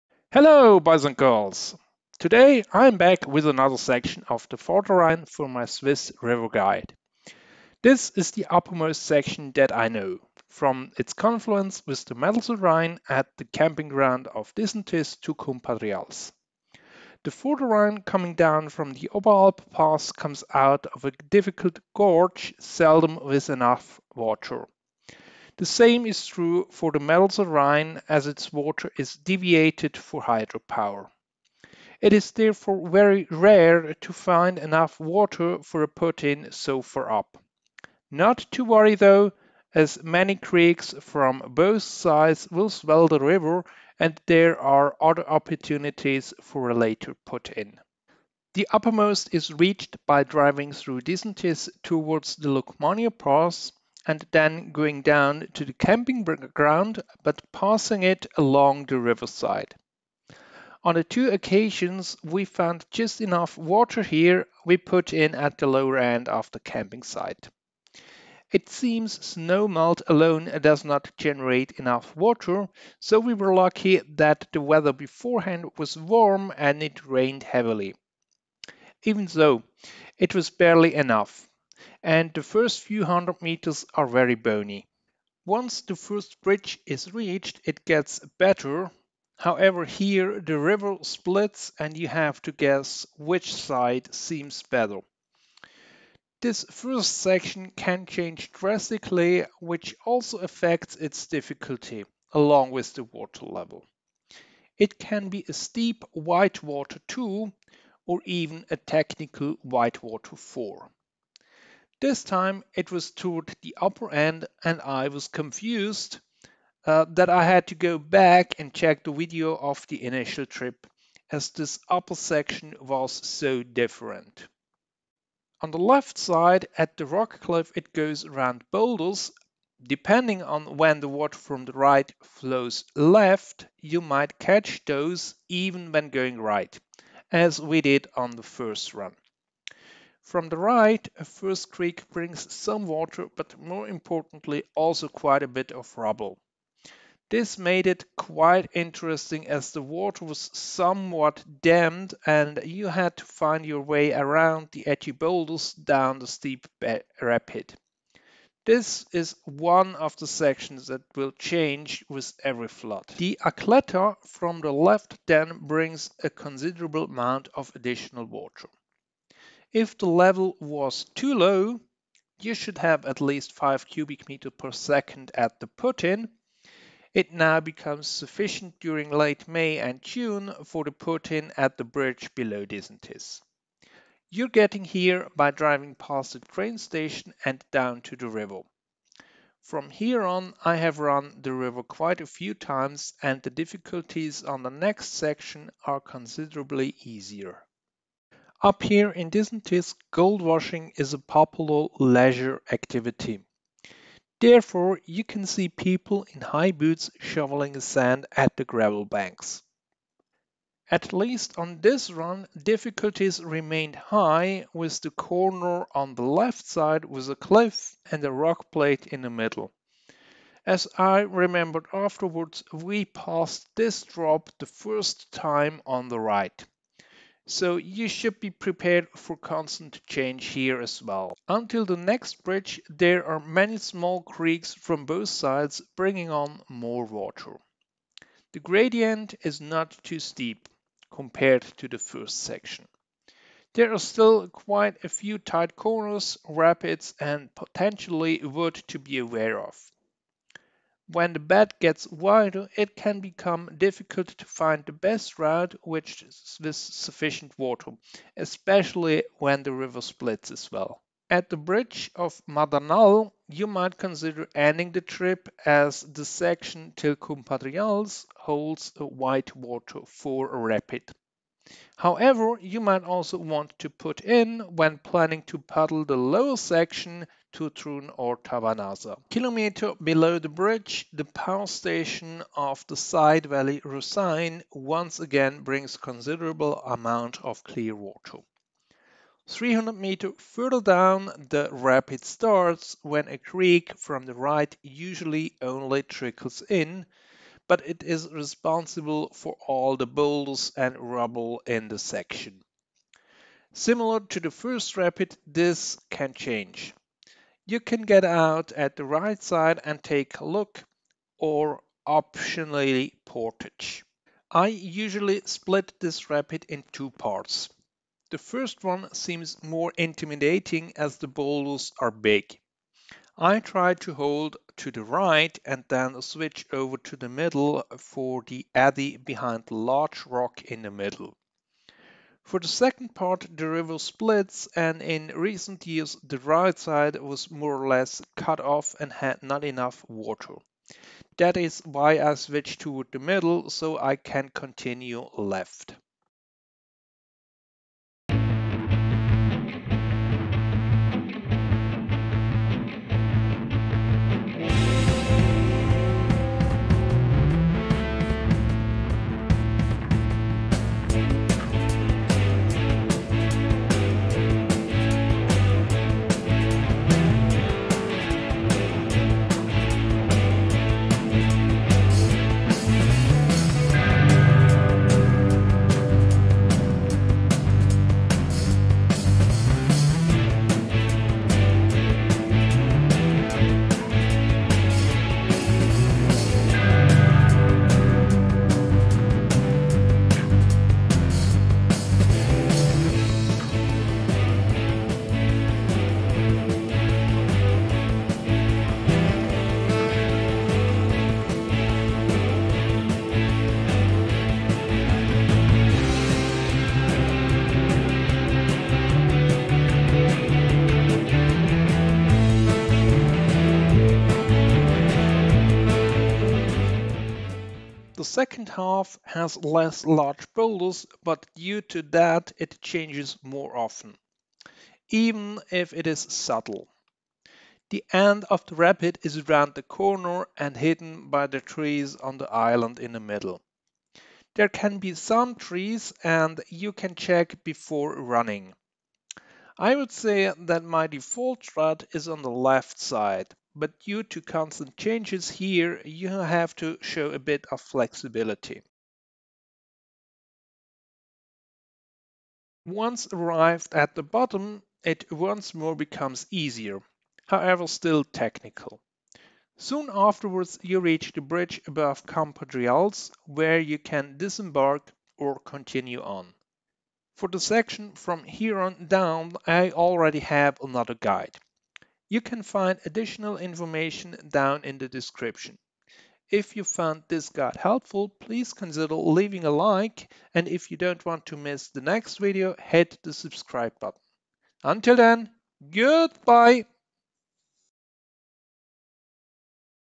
Riverguide - Vorderrhine: Diesentis - Cumpadrials
This episode of my Swiss riverguide I present the upper section of the Vorderrhine from Disentis to Cumpadrials.

Upper put in Camping Diesentis: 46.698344N, 8.858213E
Lower put in Diesentis:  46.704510N, 8.867676E
Put in/out Madernal: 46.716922N, 8.895402E
Put out Cumpadrials: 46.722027N, 8.915690E